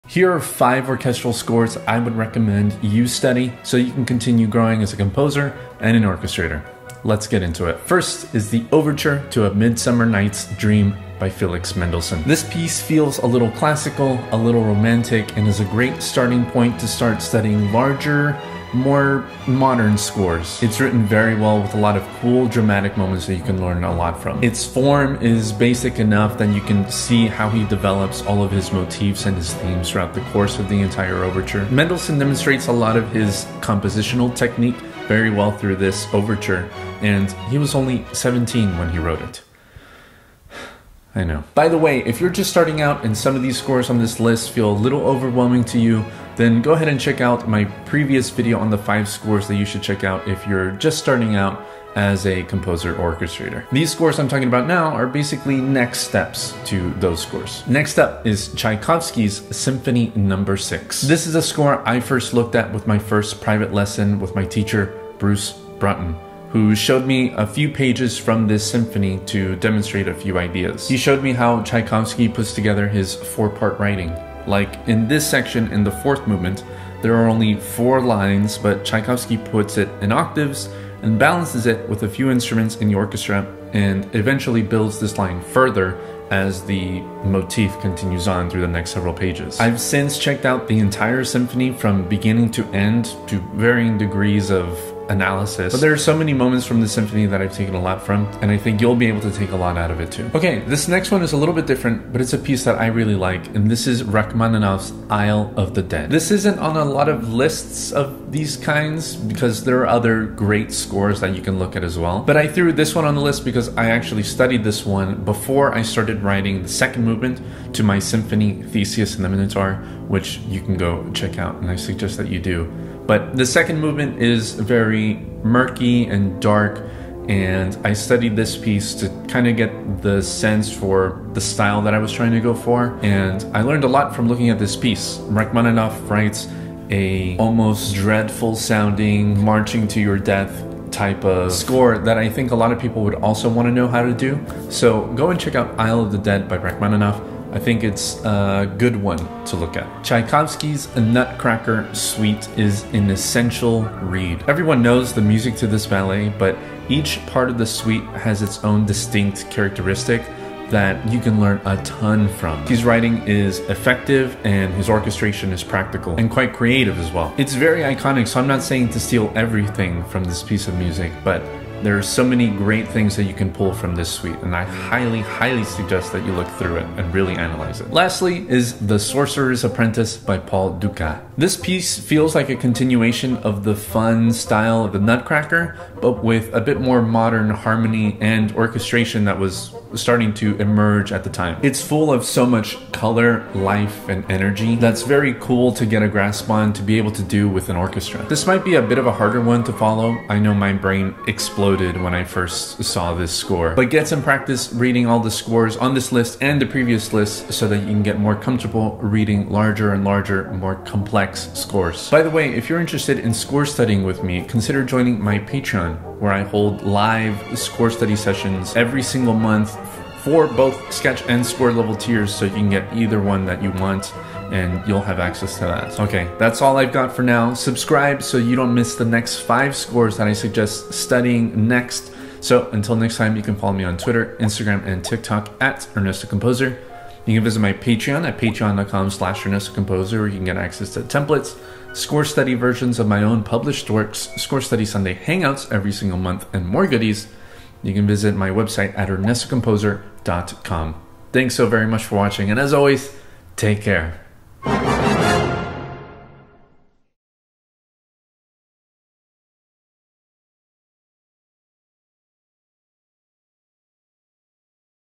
5 ORCHESTRAL Scores You Should Study Now
Here are the five orchestral scores I would recommend you study if you've already started reading scores. Check out this video for great beginner scores

▶ Check out the templates I rely on to do my work